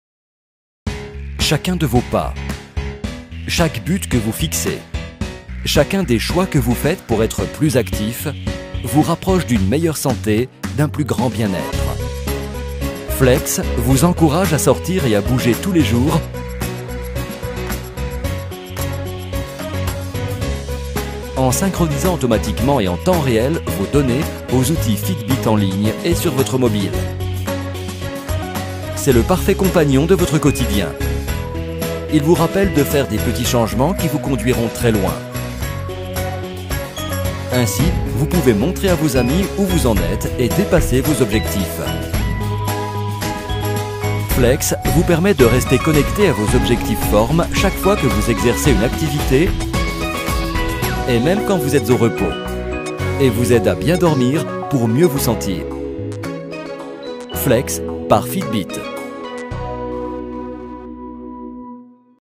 Bracelet Fitbit Flex - Vidéo officielle
Portez en permanence ce petit appareil alliant finesse et élégance. Durant la journée, il mesure le nombre de pas effectués, la distance parcourue et les calories brûlées. La nuit, il analyse votre cycle de sommeil et vous réveille en silence au matin. D'un simple coup d'œil sur les voyants, vous savez où vous vous situez par rapport à votre objectif personnel. Il est le moteur dont vous avez besoin pour vous motiver à sortir et à être plus actif.